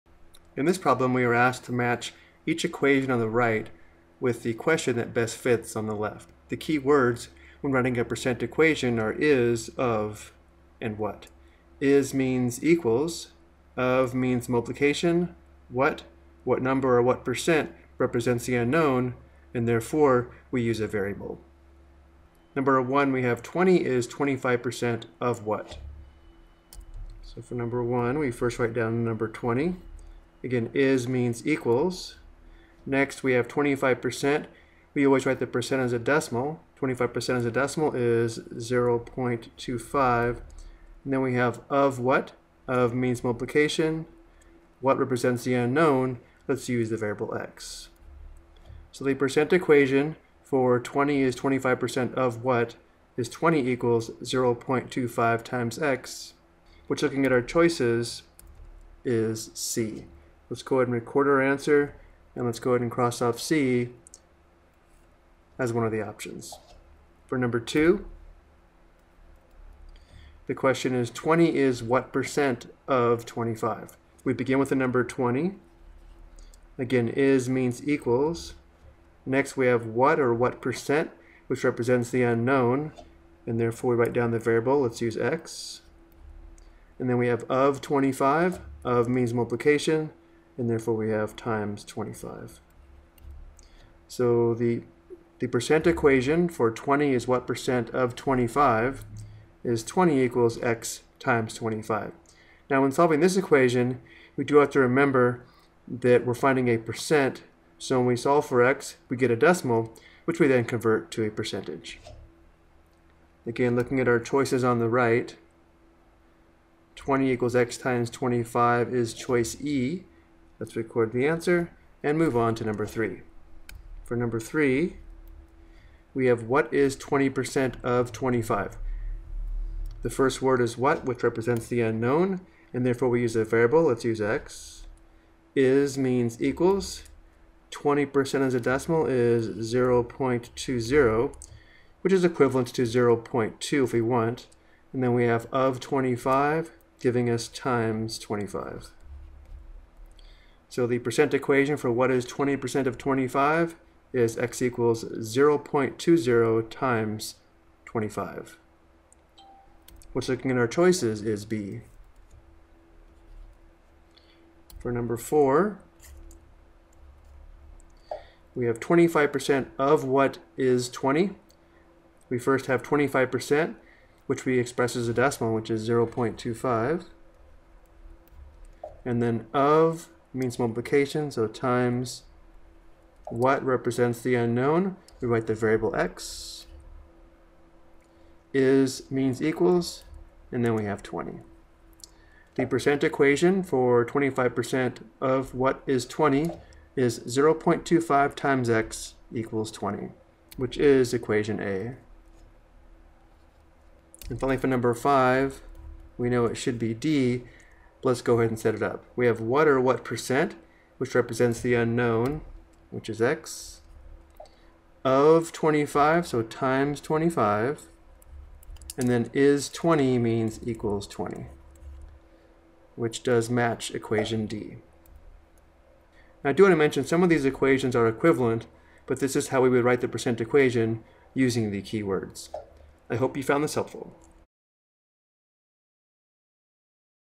The Percent Equation: Matching Questions to Percent Equations
The video explains how to write percent equations from questions.